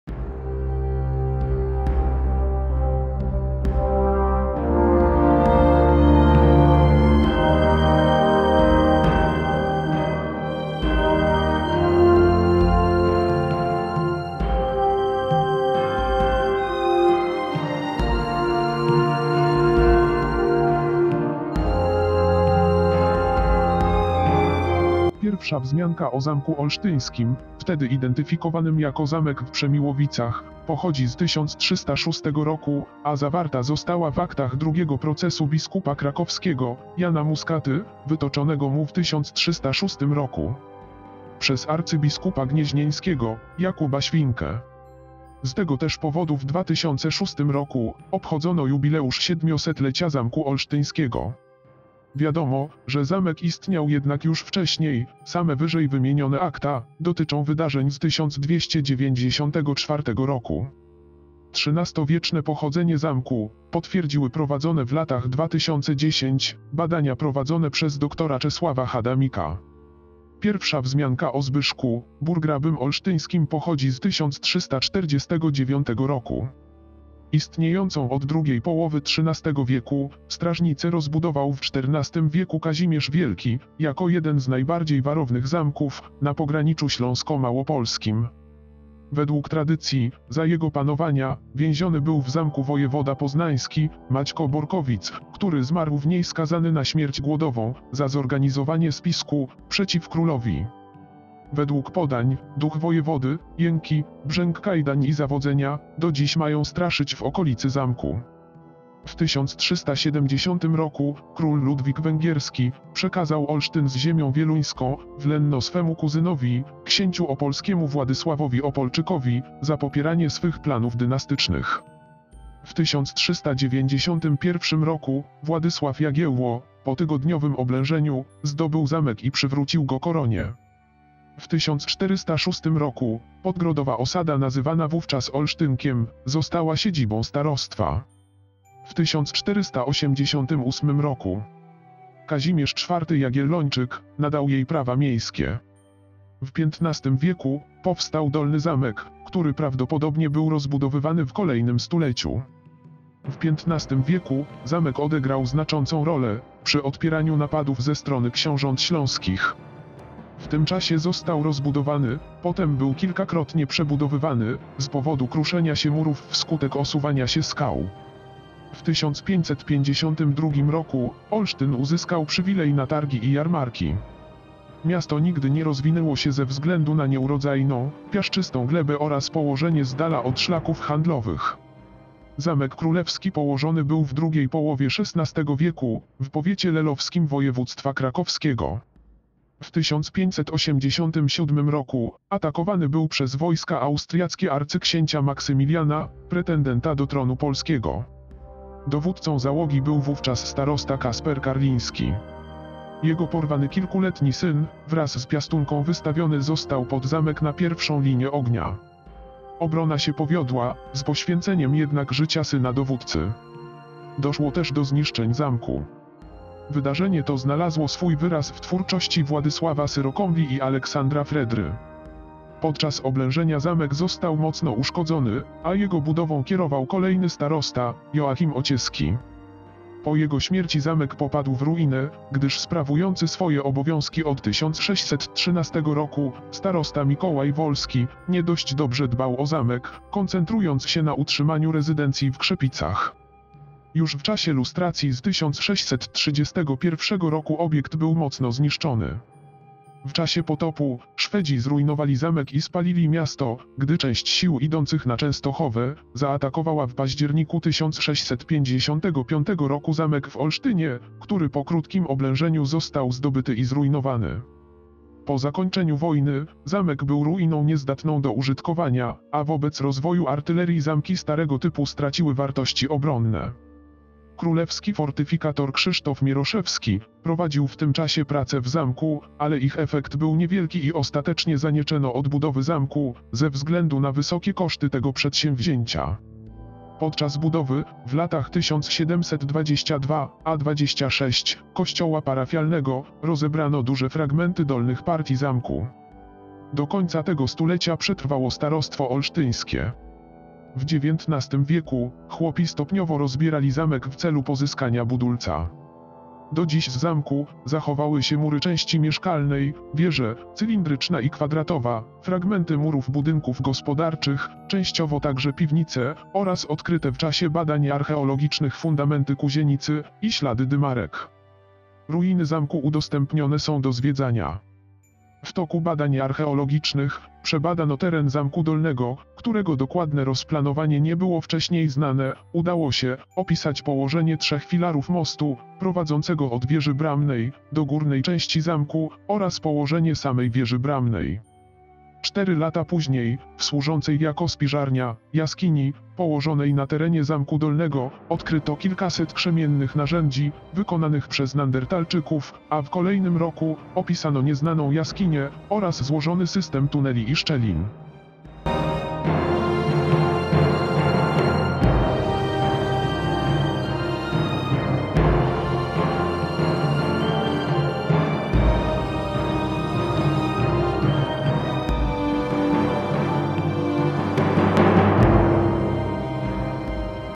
Zamek w Olsztynie #20 (Województwo śląskie)
Zamek w Olsztynie, ruiny zamku leżącego na Jurze Krakowsko-Częstochowskiej, wybudowanego w systemie tzw. Orlich Gniazd, we wsi (dawniej mieście) Olsztyn w województwie śląskim, w powiecie częstochowskim.